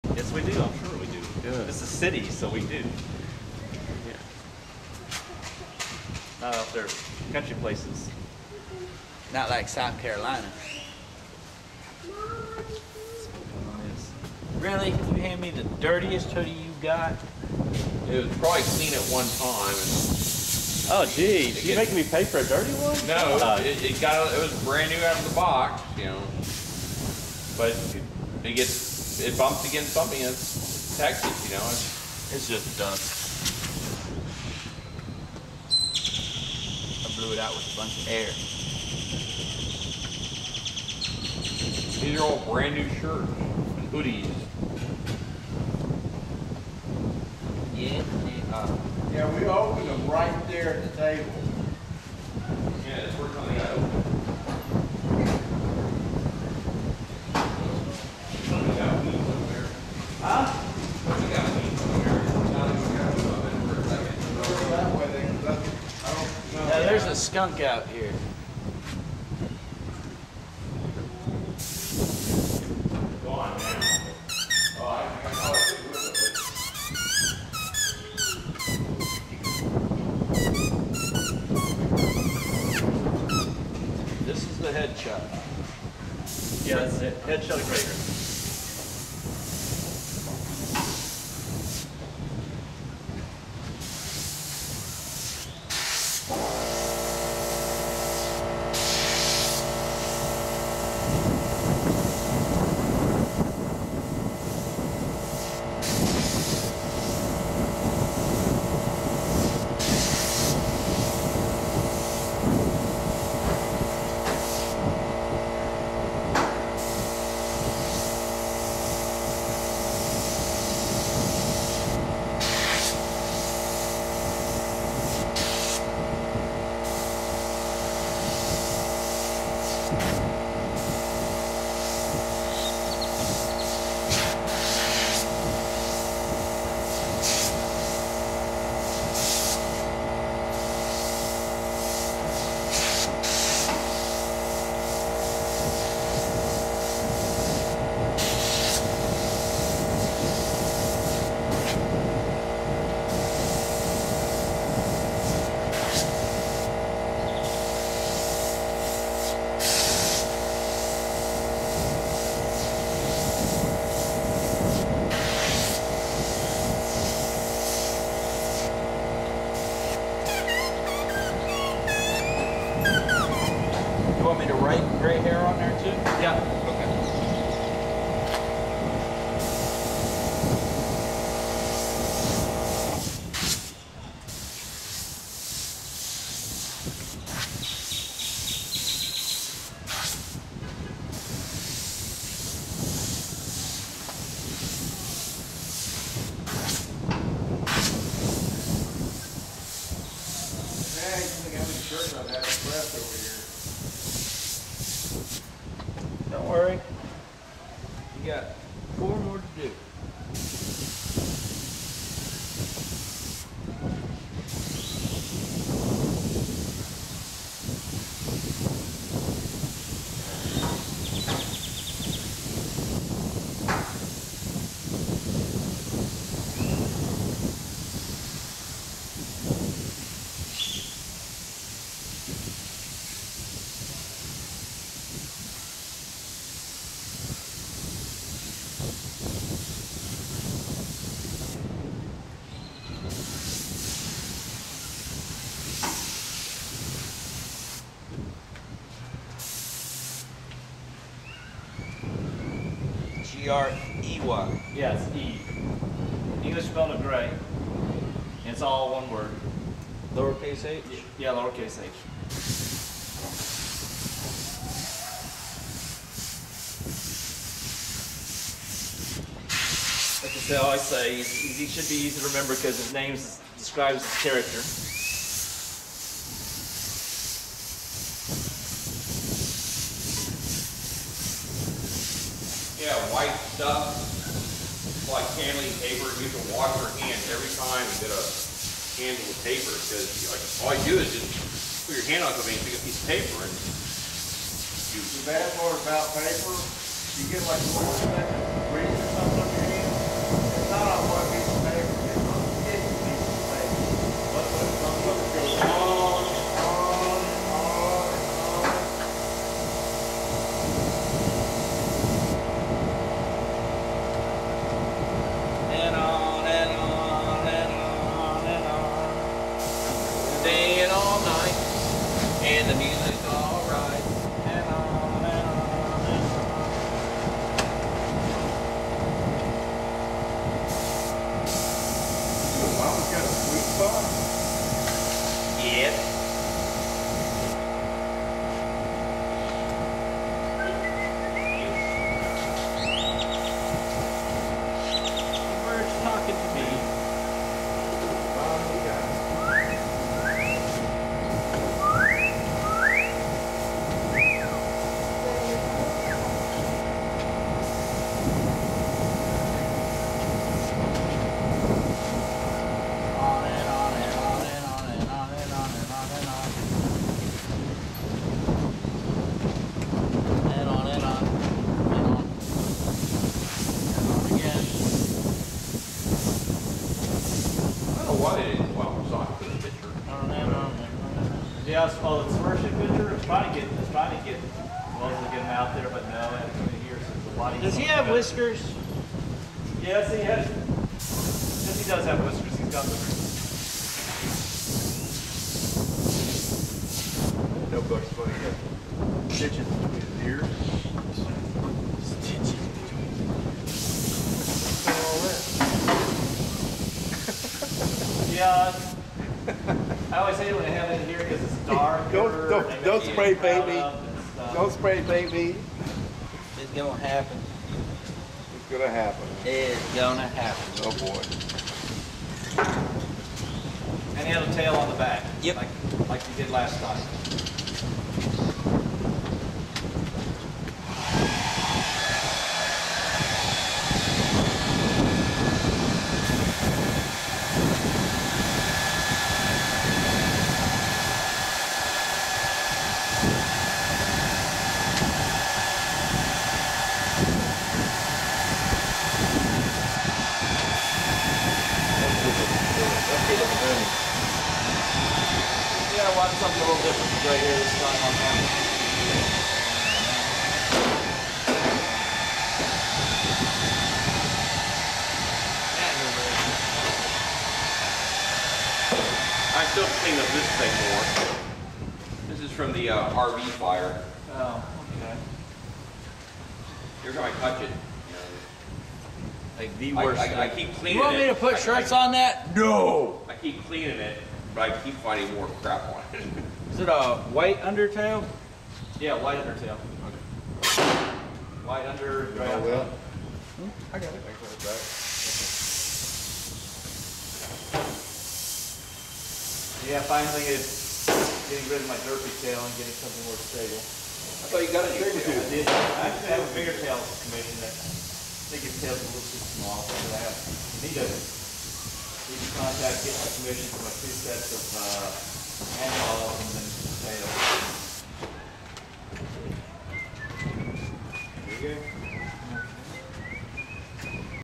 Greyhare Hoodie Air Brush
This is an air brush hoodie of Greyhare done by Nash the Panda. This was done at the Arlington Fur Meet on Mother's Day 5/14/2016.